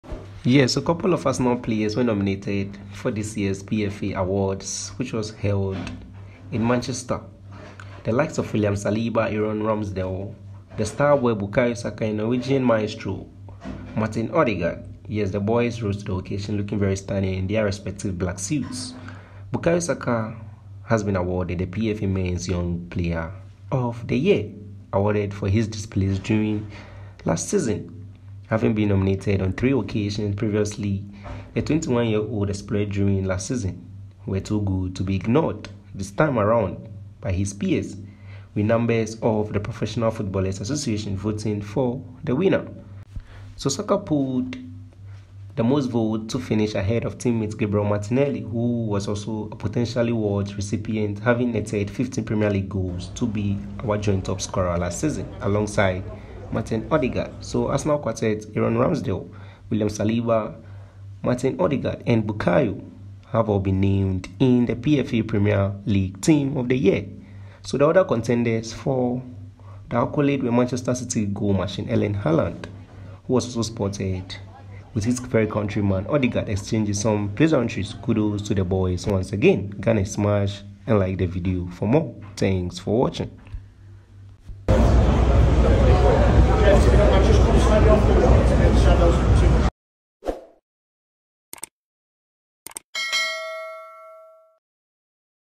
👏Congrats!Bukayo Saka Wins PFA Young Player’s Awards,As Saliba,Ramsdale & Odegaard Made EPL Teams.
So couple of arsenal players were nominated for this year’s PFA awards which was hosted in Manchester. The likes of William Saliba, Aaron Ramsdale, The star boy Bukayo Saka and the Norwegian maestro Martin Odegaard. Yes,the boys rose to the occasion looking very stunning in their respective black suits. Bukayo Saka has been awarded the PFA men’s Young Player of the Year award for his displays during the 2022/23 season
Having been nominated on three occasions previously, the 21-year-old’s exploits during last season were too good to be ignored this time around by his peers, with members of the Professional Footballers’ Association voting for the winner. So Saka pulled the most votes to finish ahead of teammate Gabriel Martinelli, who was also a potentially worthy recipient having netted 15 Premier League goals to be our joint-top scorer last season alongside Martin Odegaard. Arsenal quartet Aaron Ramsdale, William Saliba, Martin Ødegaard & Bukayo Saka have all been named in the PFA Premier League Team of the Year. The other contenders for the accolade were Manchester City goal machine Erling Haaland. Who was also spotted with his very country man , Martin Odegaard exchanging pleasantries.  Kudos to the boys once again, Gooners Smash the subscription button and be here for more.

PFA Awards
Bukayo Saka PFA Awards
Bukayo Saka News